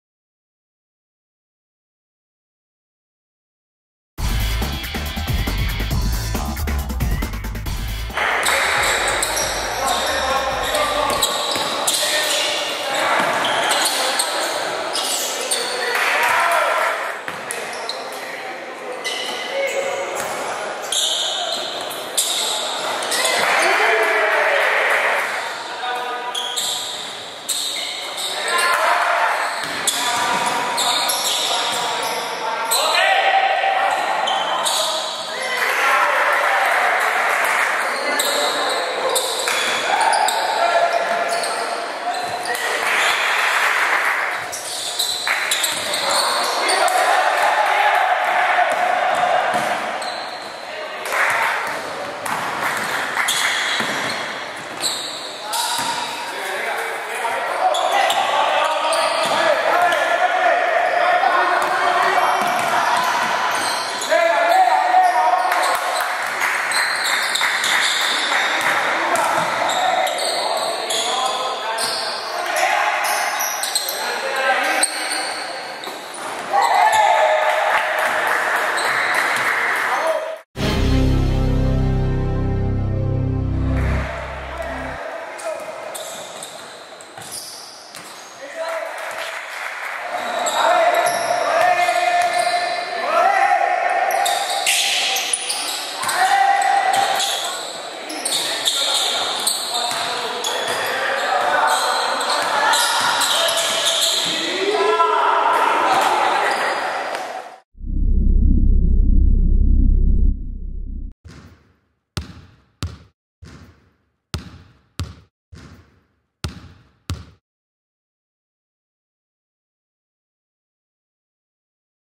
Lajas Sub 22 vs Sabana Grande
Partido entre Los Cardenales de Lajas Sub 22 y Sabana Grande. Victoria para Lajas Sub 22 79-60 sobre Sabana Grande. Josue Santiago anoto 33 puntos en el partido.